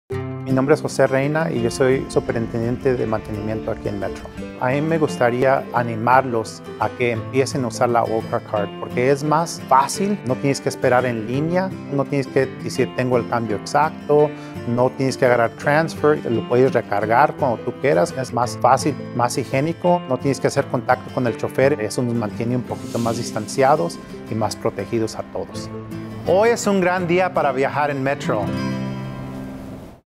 Use ORCA when you return to King County Metro
It’s a great day to ride Metro with your ORCA card. Metro is safe and ready for its passengers, and ORCA cards make boarding the bus faster, easier, and more germ-free than using cash. To get your ORCA card, go to ORCA Savings.com/viaje.